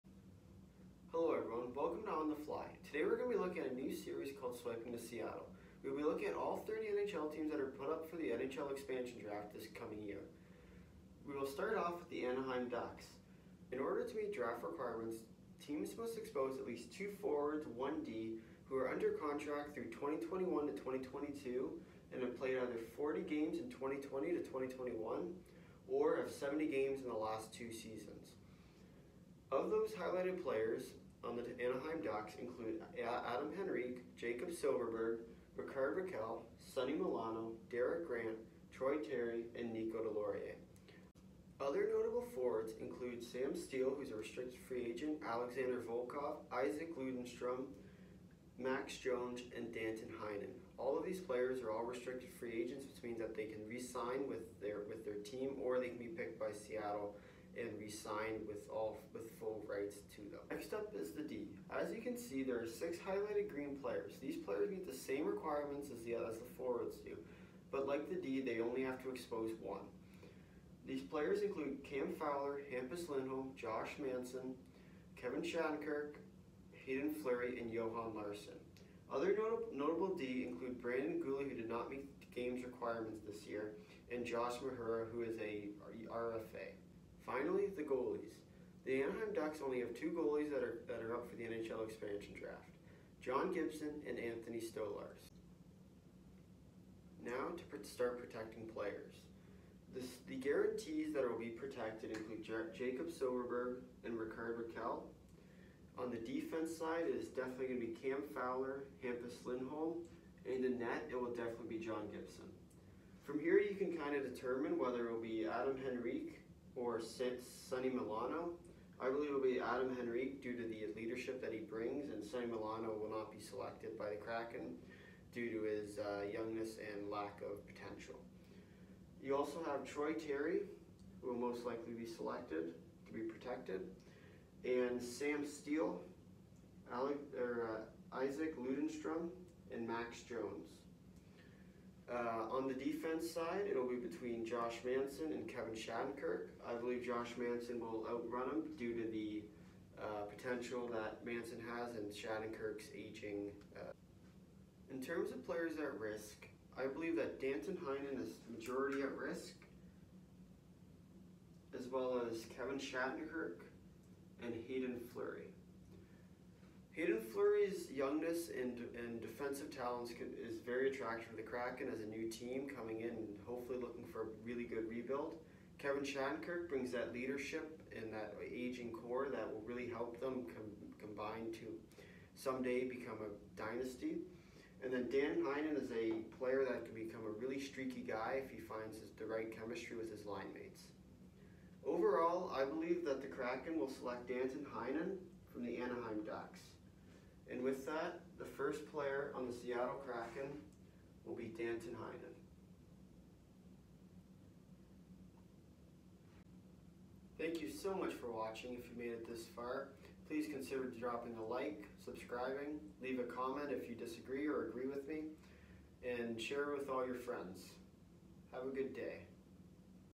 Swiping to Seattle: Anaheim Ducks
Welcome to On The Fly! Swiping to Seattle is a 30 part series that will go in-depth into the upcoming expansion draft. This will be your one stop shop for everything hockey!

Please consider giving the video a like and drop a sub if you really enjoyed it.

Much Appreciated!